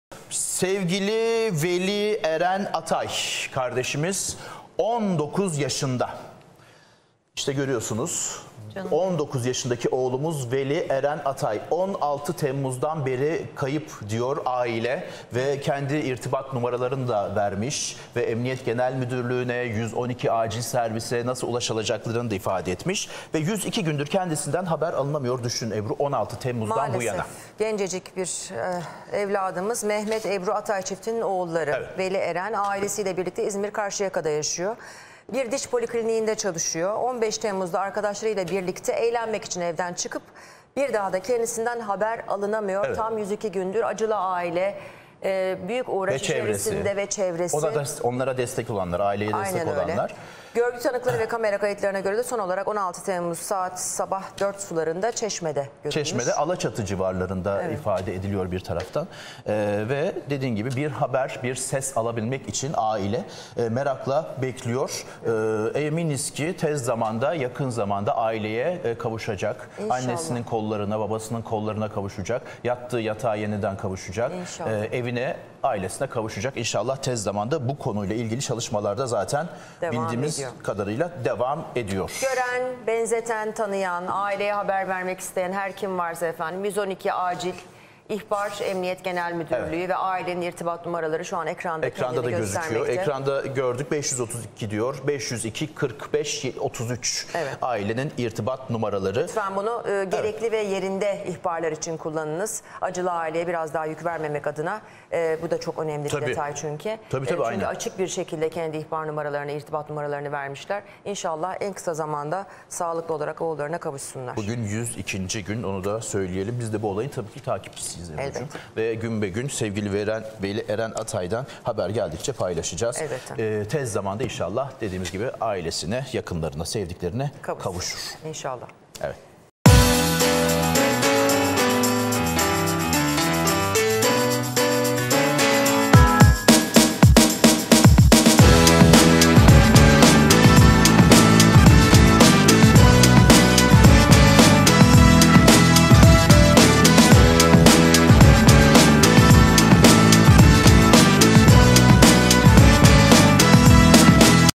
Veli Eren Atay 102 gündür kayıp!
Veli Eren Atay'dan 102 gündür haber alınamıyor... Ailesi ondan alacakları bir haber bekliyor!